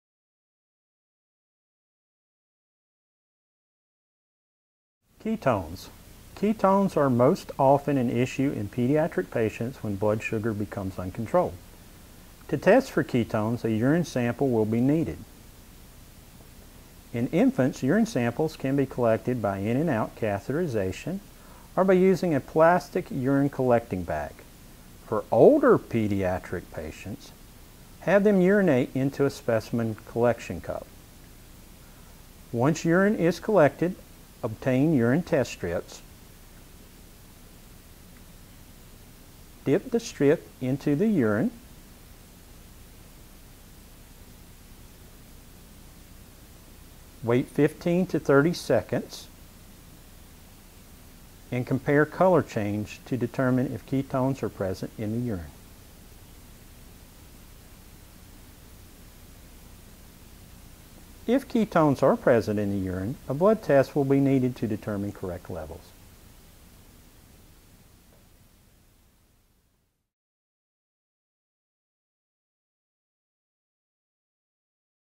Testing Ketones MP4
Short film by East Carolina's College of Nursing's Concepts Integration Lab. Proper way to correctly test Ketones.